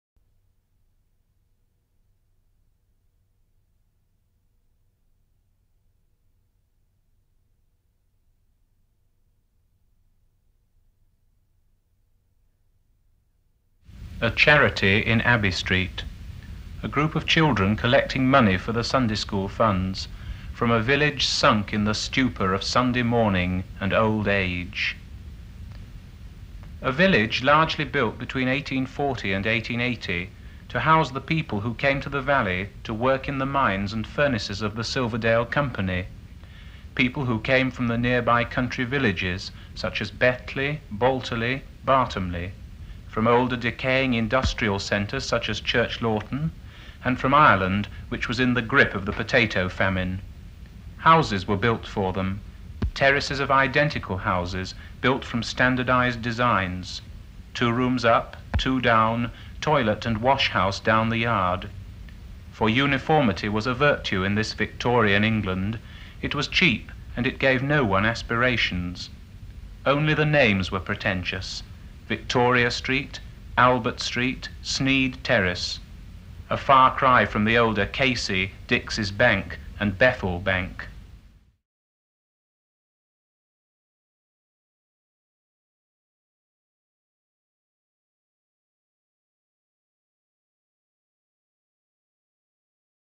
Silverdale 1964
DeA Charity in Abbey Street – a group of schoolchildren collecting money for Sunday School funds – from a village sunk in the stupour of Sunday morning and old age. A village largely built between 1840 and 1880 to house the people that came to the valley to work in the mines and furnaces of the Silverdale Company. People who came from nearby country villages such as Betley, Balterly and Barthomley. From older decaying industrial centres such as Church Lawton and from Ireland – which was in the grip of the potato famine. Houses were built for them. Terraces of identical houses, built from standardised designs. Two rooms up, two down – a toilet and wash house down the yard. For uniformity was a virtue in this Victorian England. It was cheap and it gave no one aspirations. Only the names were pretentious; Victoria Street, Albert Street, Sneyd Terrace. A far cry from the older Casey, Dixie’s Bank and Bethel Bank.scription